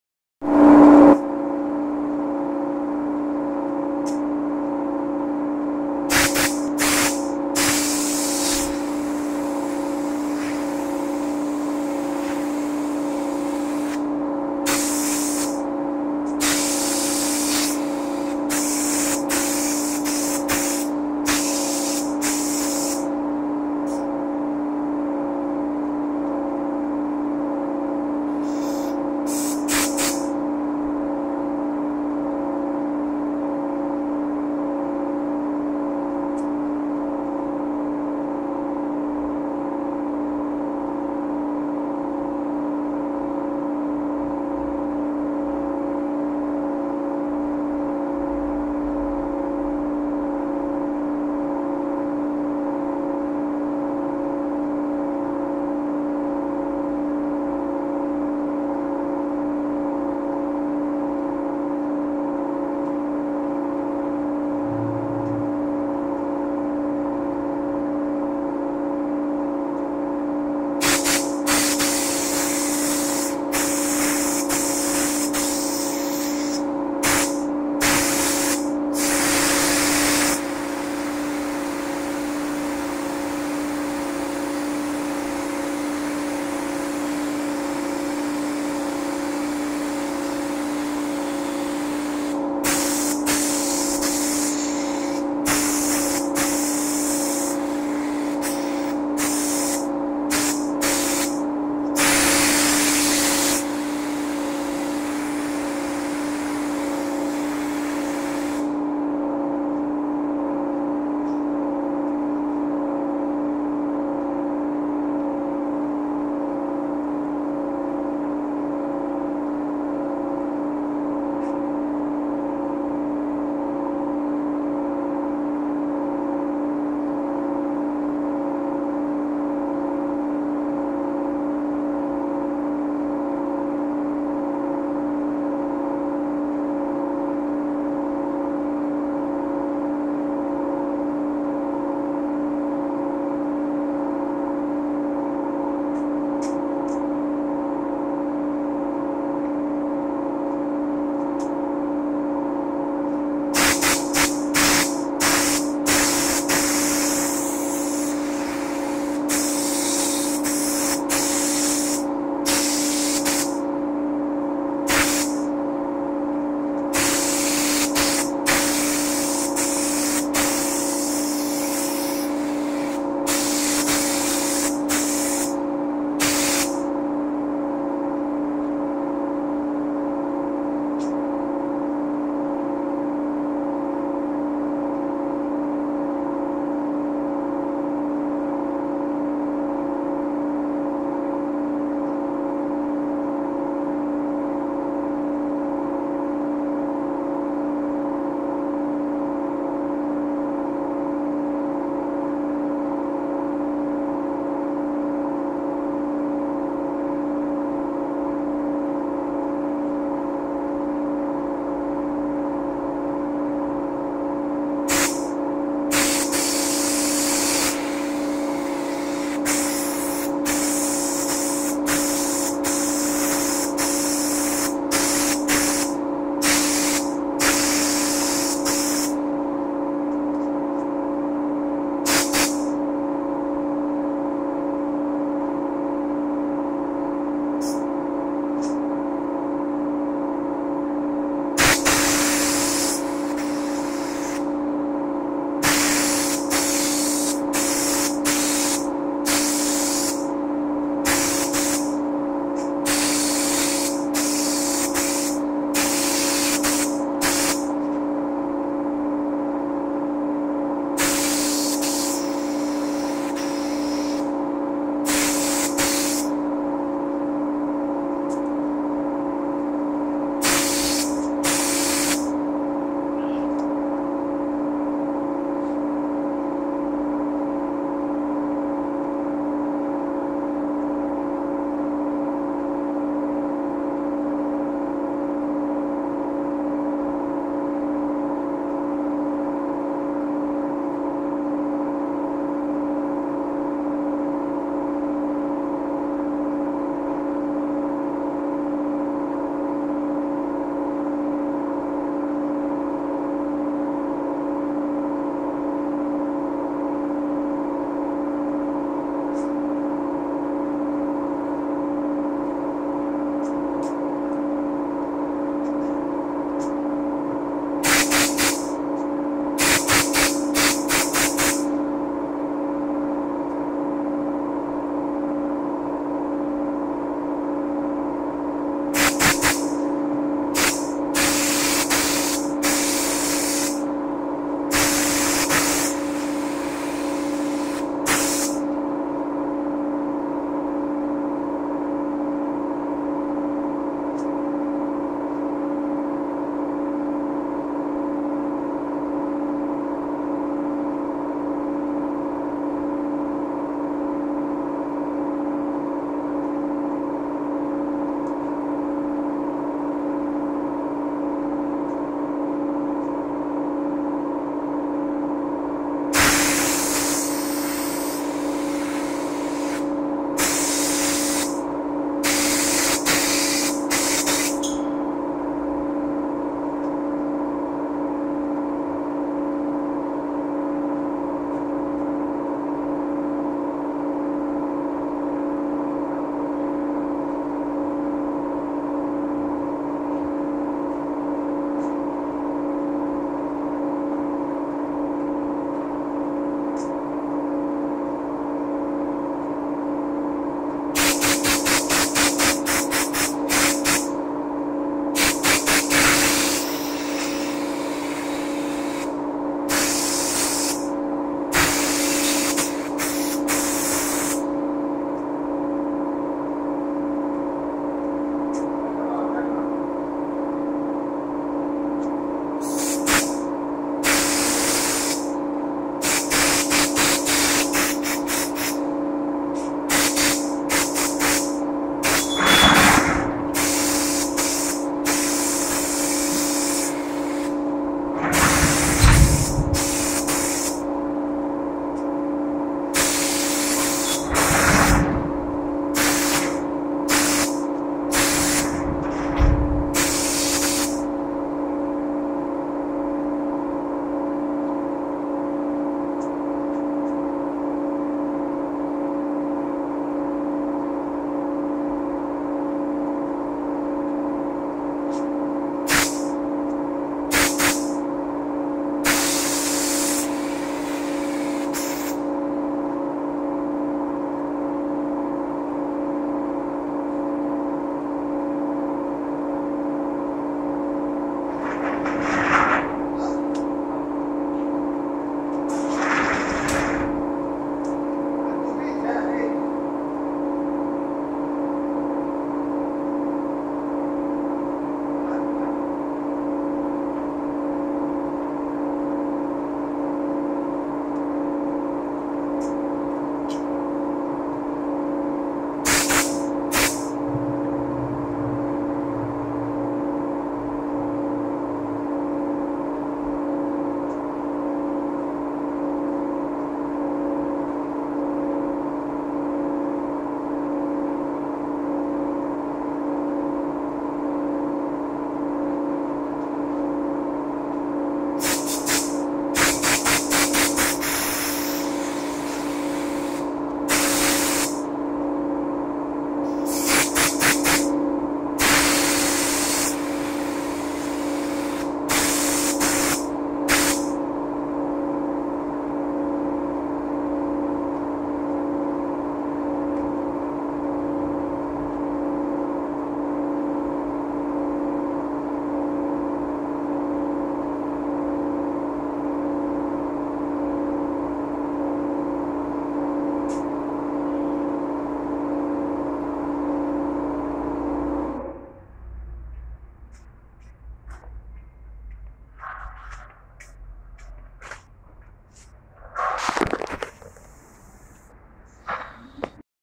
Top coating mirror effects(1)
Spraying a top coat glass effects and how to laern a very simple ang especial stroke by doing an spray style ang stroke by bongc tv